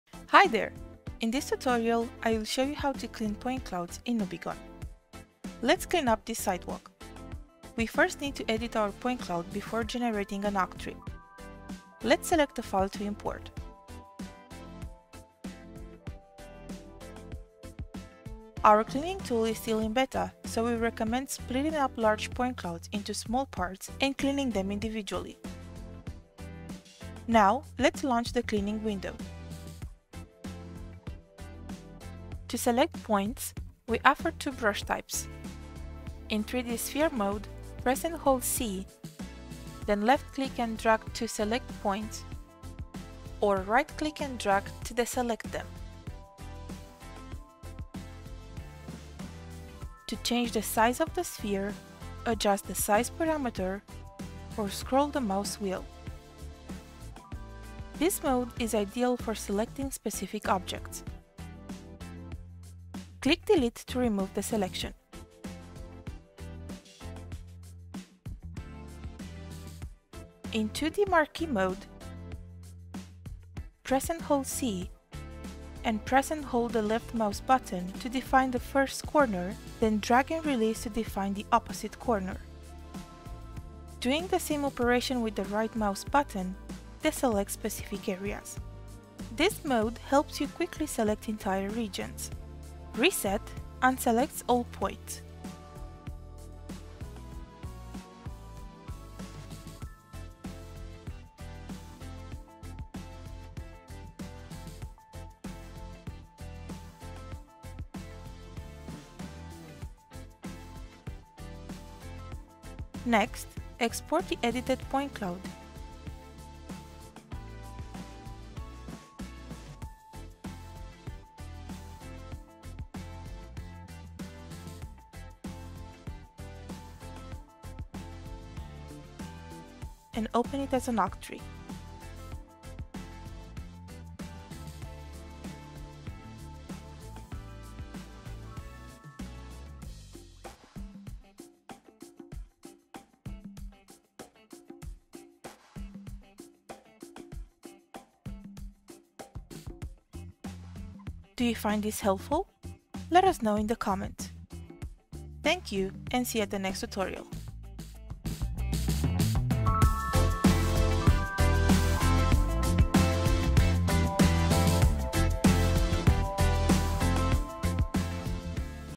How to clean point clouds
NUBIGON Pro is a point cloud visualization and animation software that helps users efficiently engage with datasets, combine deliverables, and produce flythrough videos.